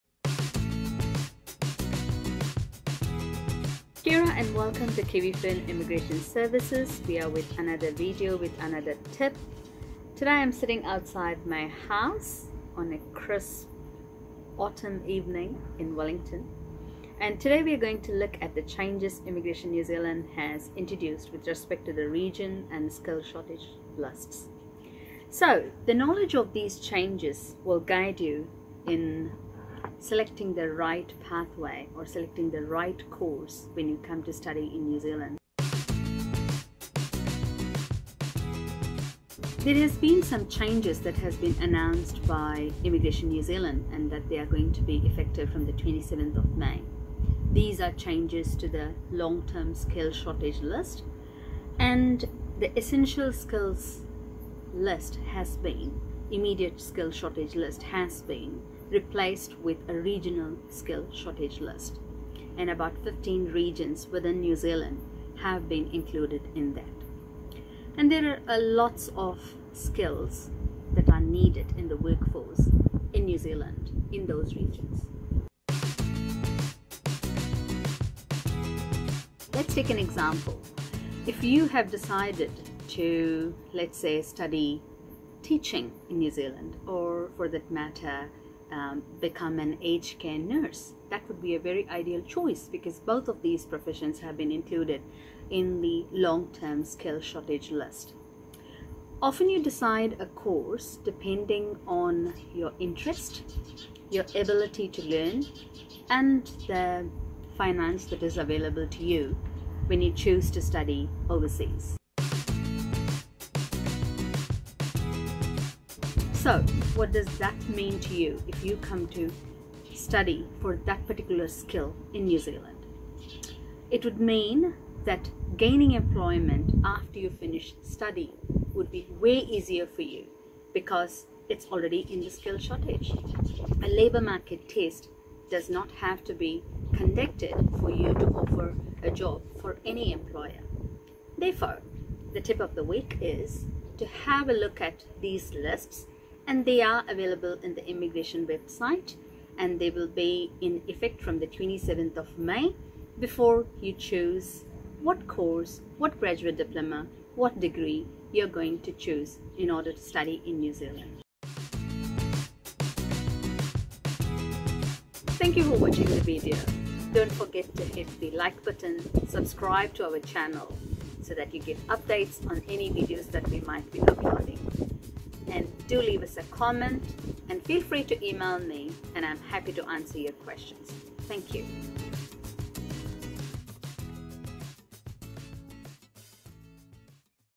Considering the New Zealand Regional Skill Shortage Lists
Tipoftheweek: Considering the New Zealand Regional Skill Shortage Lists

Kia ora everyone! This week we take a brief look at the changes to the Essential Skills in Demand programme - particularly the introduction of the Regional Skill Shortage Lists. These lists should be taken into account if you are migrating to New Zealand to study with a future focus on employment in your chosen field.

Please remember that every case is different and applications are assessed on their own merit.